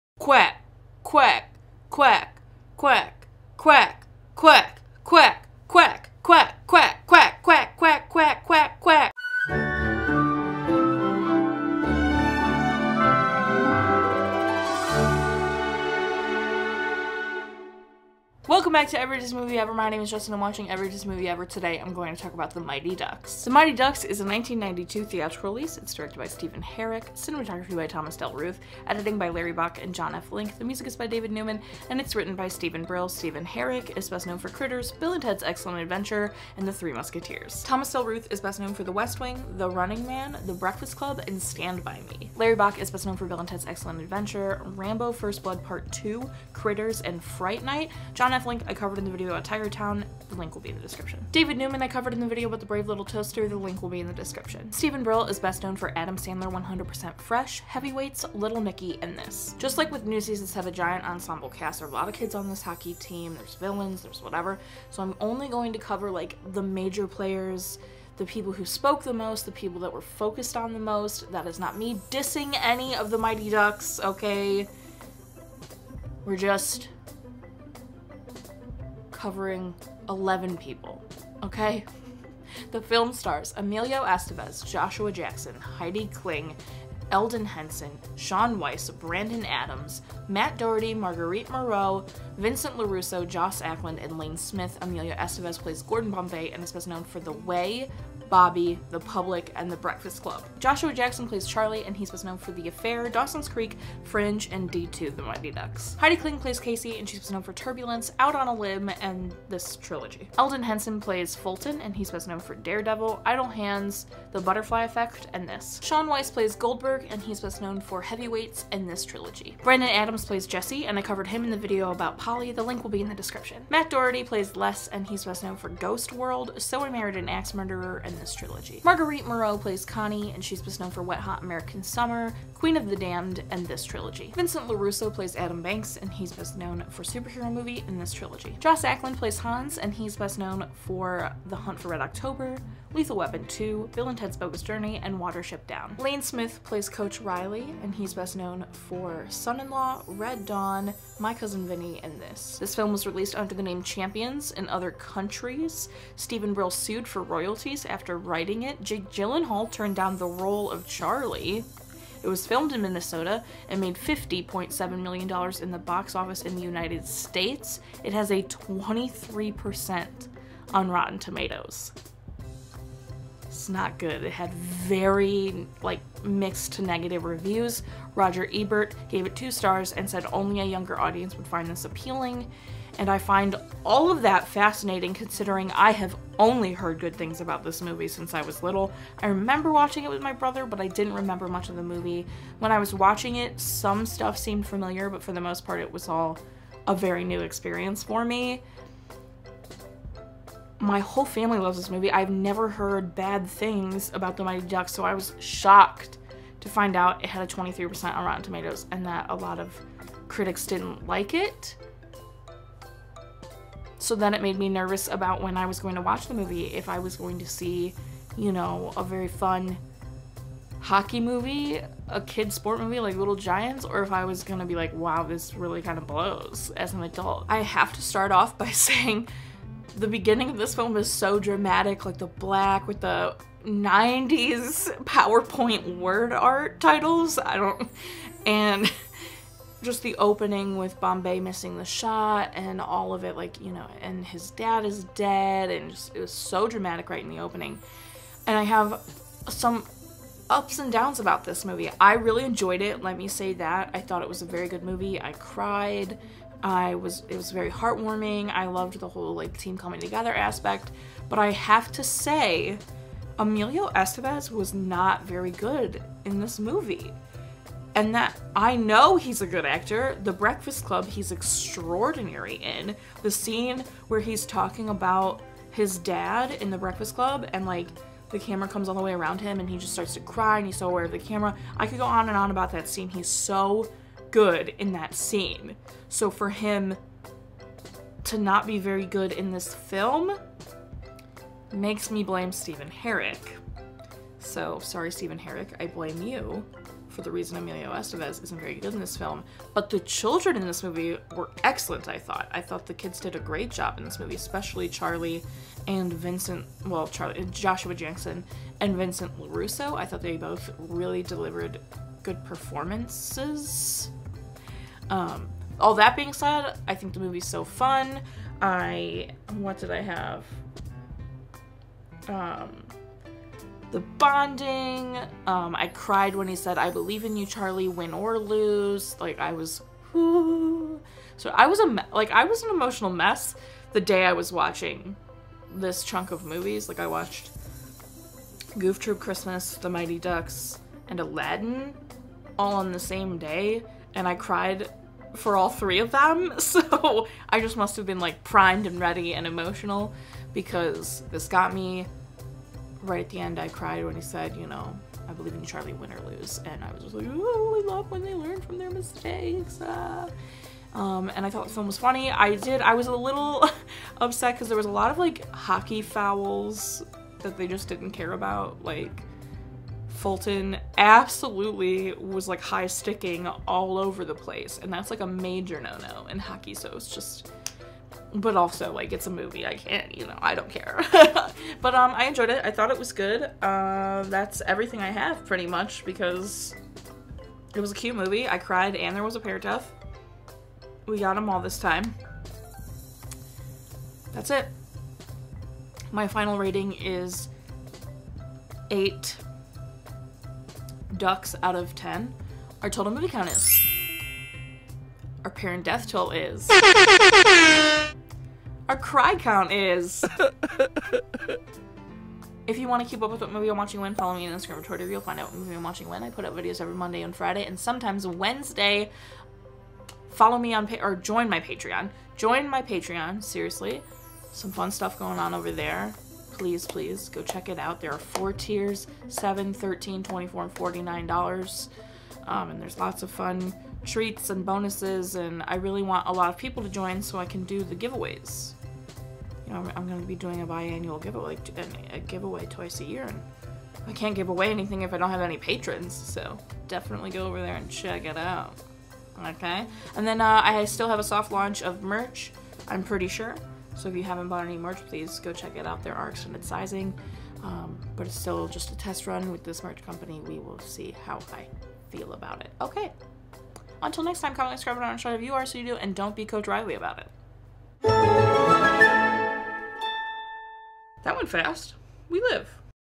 Every Disney Movie Ever: The Mighty Ducks
A couple things! First, I used duplicate pictures because those were all I had for those specific people. Second, I forgot to put the clip of Brandon Adams in Polly so don't come for me. He was in Polly.

The Mighty Ducks is a 1992 theatrical release.

Director: Stephen Herek
DP: Thomas Del Ruth
Writer: Steven Brill
Lead Actor: Emilio Estevez, Joshua Jackson, Heidi Kling, Elden Henson, Shaun Weiss, Brandon Adams Matt Doherty, Marguerite Moreau, Vincent LaRusso, Joss Ackland and Lane Smith

TOTALS:
Movies watched: 338
Parent Deaths: 87
Cry Count: 35

RATINGS:
Directing: 7
Cinematography/Animation: 8
Editing: 8
Writing: 7
Acting: 8
Opinion/Enjoyment: 8
FINAL RATING: 8

THANK YOU TO PATRONS:
- Mariah Thomas